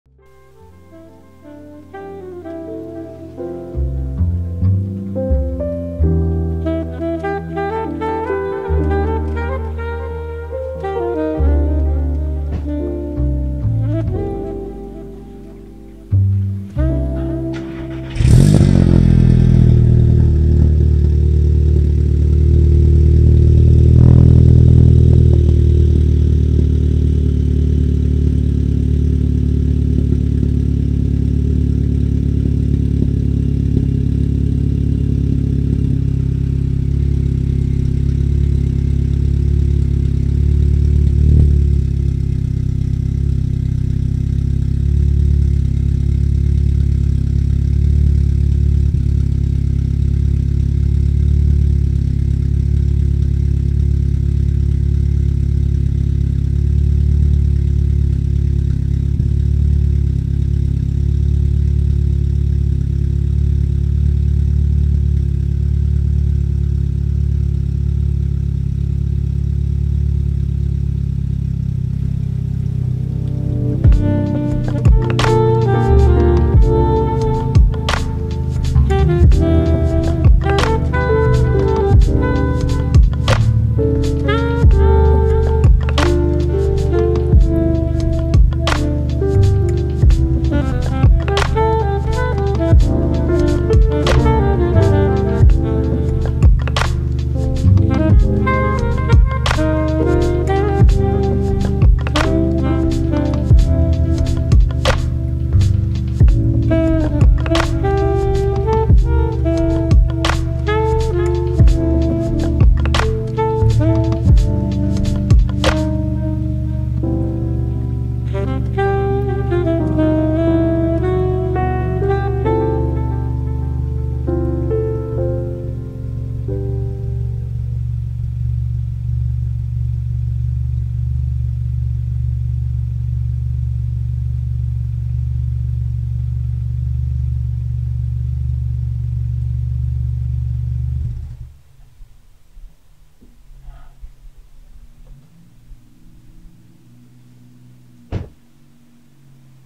MK7 GTI Milltek Resonated Cold Start | Cinematic Flair Enhanced
Subtle cold start to idle on a vortex'd MK7 VW GTi